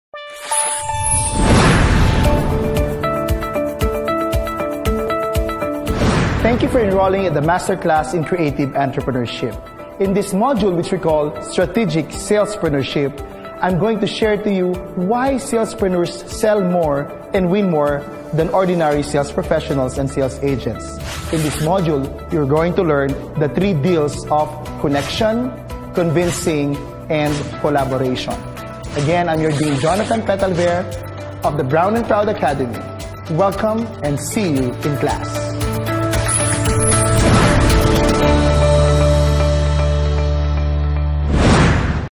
Module 3: How to Sell More Remotely
The 3 chapters of this module will teach us how to become sales strategists - key secrets while business owners sell and win more versus sales professionals. Each learner will understand the 3Cs of Strategic Salespreneurship - CONNECTION - CONVINCING - COLLABORATION.

to get FREE Entrep Learning Materials